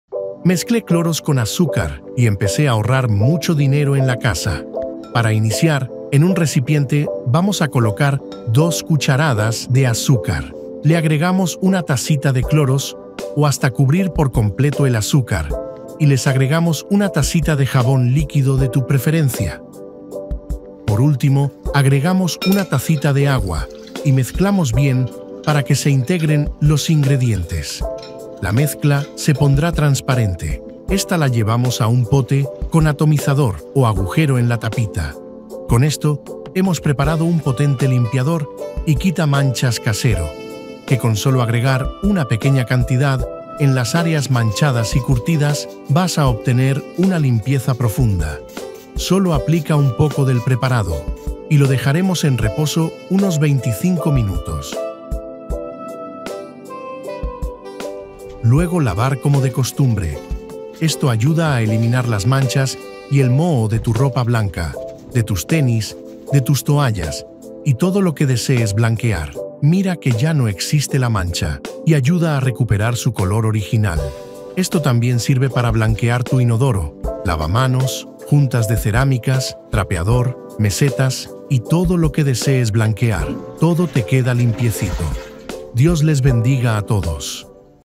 Esto Elimina la grasa y el cochambre del extractor, la estufa, ollas, fregaderas y mas
Elimina la grasa y el cochambre del extractor, la estufa, ollas,  fregaderas y mas